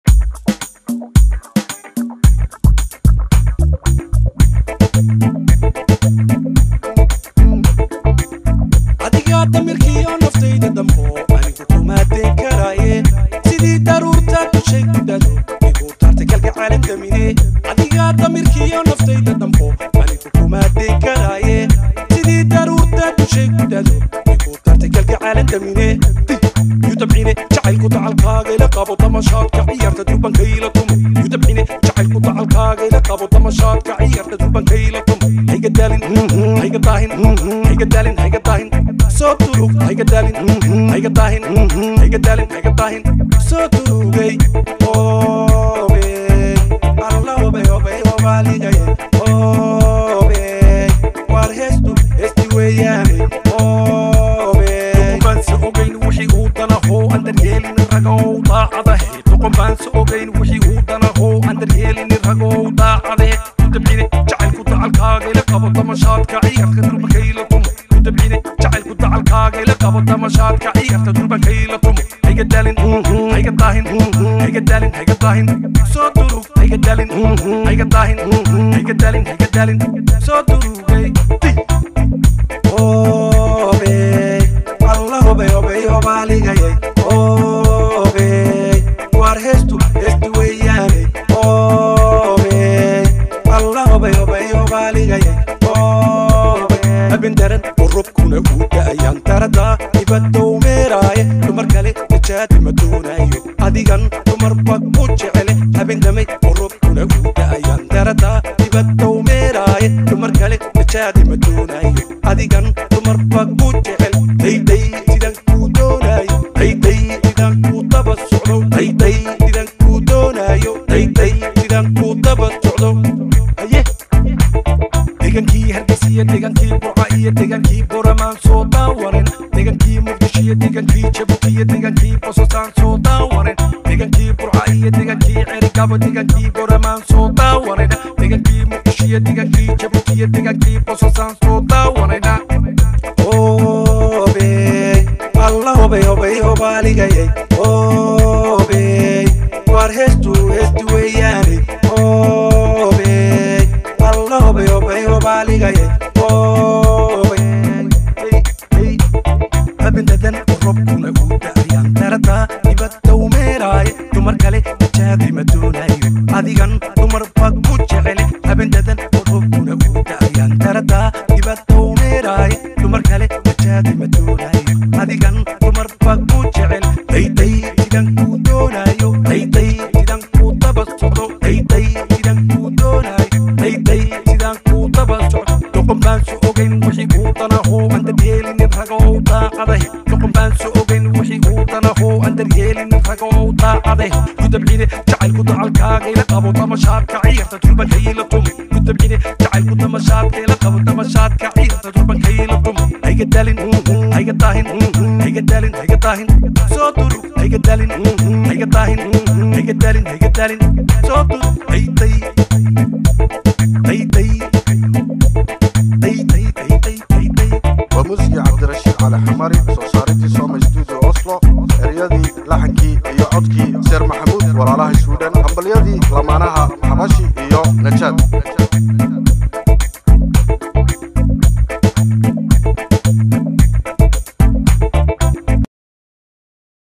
Sir Mohamud Soma Studio ( Hees cusub)
Waar heestu heestii weeyaane , Sir Mohamud wuu ku farayaraystay heestan, waa mirihii ioyo codkii  Sir Mohamud.
Music Abdirashid A Xamari. ( Soma Studio Oslo).